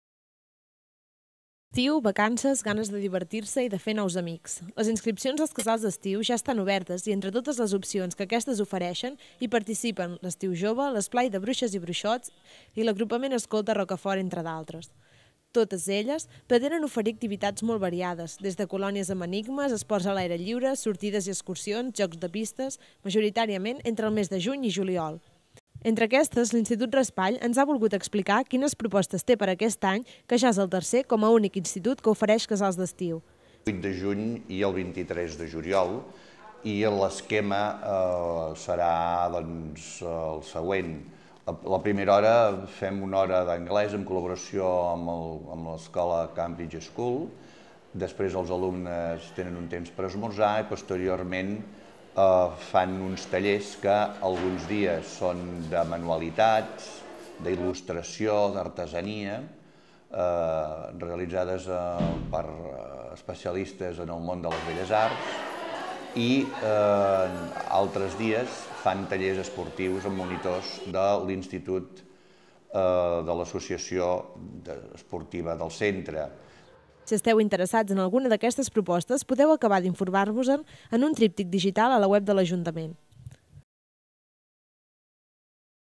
Casals també a l'institut
Sembla que lestiu cada cop és més aprop i és que les activitats destiu i casals que es proposen per a lestiu de molts nens i nenes ja van obrir inscripcions fa dues setmanes. Entre totes aquestes propostes hi trobem, per tercer any, lúnic institut que ofereix aquest tipu dactivitats, el Raspall.